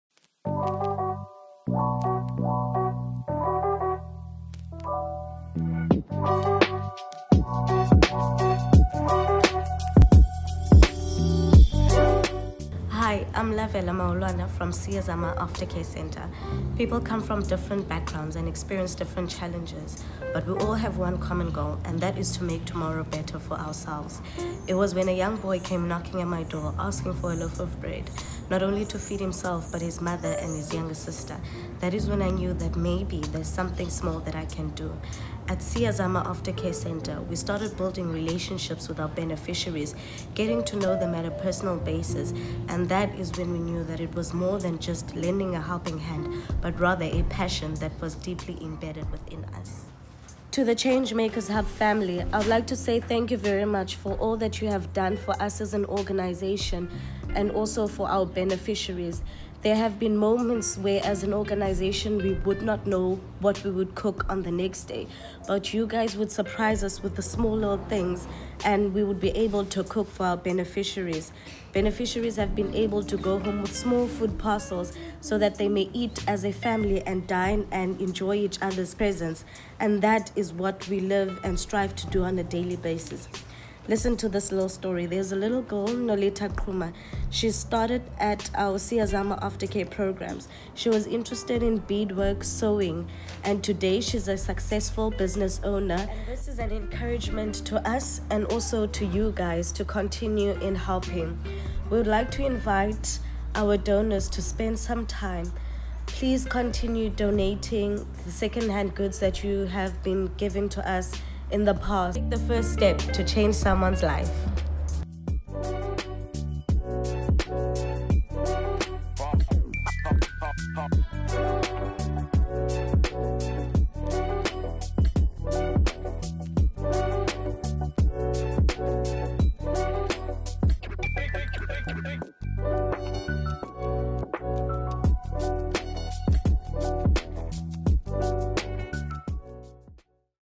Siyazama Aftercare Centre
Siyazama Aftercare Centre is a community-driven outreach project, established in 2000. It is a non-profit organisation formed by a group of women who have combined their various skills and resources and engaged in various projects with a view to empower learners and youth with skills to make them more employable.